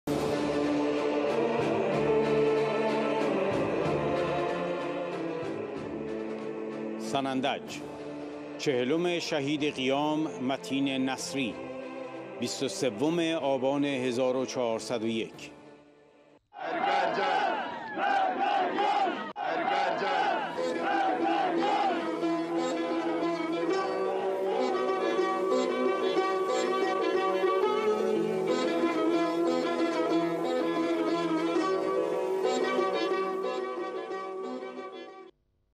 سنندج چهلم شهید قیام متین نصری - ۲۳آبان۱۴۰۱
سنندج ـ ۲۳آبان۱۴۰۱
مردم سنندج در چهلم شهید قیام متین نصری بر سر مزار او حاضر شدند و شعار مرگ بر جاش سردادند.

سیمای آزادی ۲۳آبان ۱۴۰۱